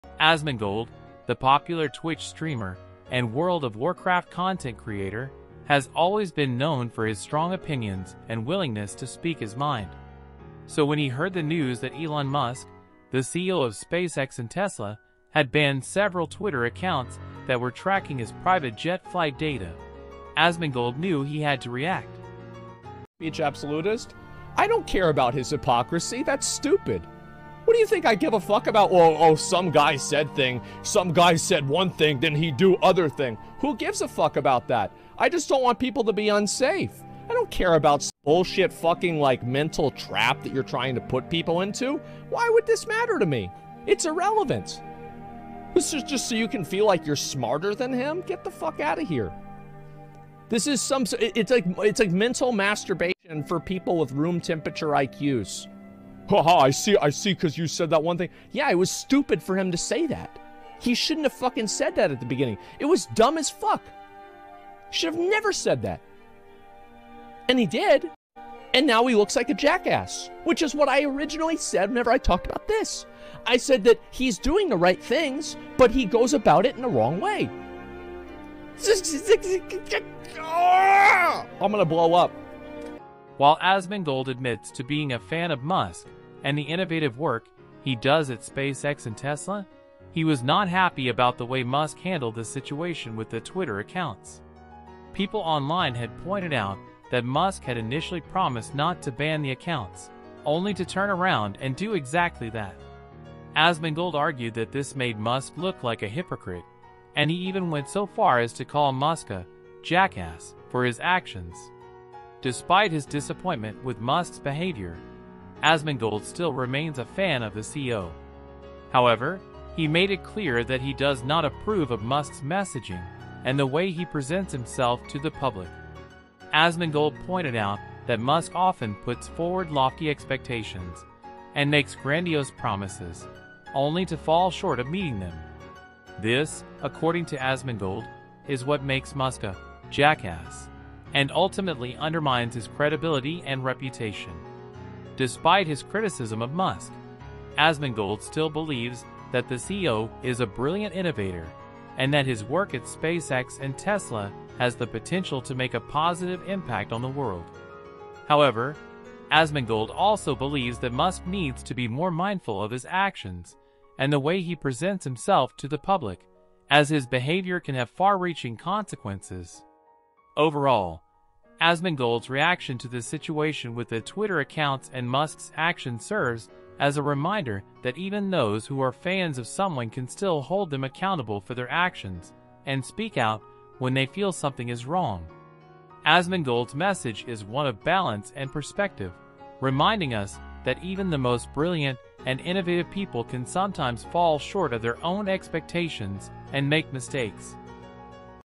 Asmongold Reacts To Elon Musk Mass Banning Twitter Accounts Of Journalists
IF YOU WANT YOUR BEATS AND SONGS ON SPOTIFY AND ALL MUSIC STREAMING PLATFORMS